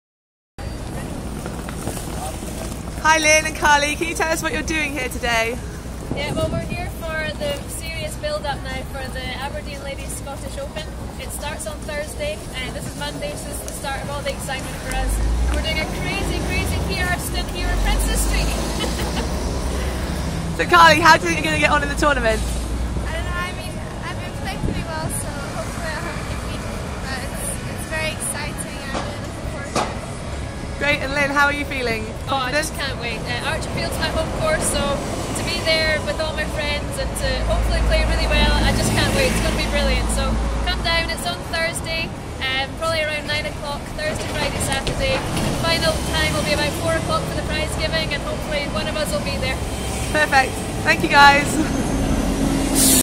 Lynn Kenny & Carly Booth blog from Edinburgh City centre
Just three days before the Aberdeen Ladies Scottish Open, Aberdeen ambassadors Lynn Kenny and Carly Booth took to the streets of Edinburgh to promote the tournament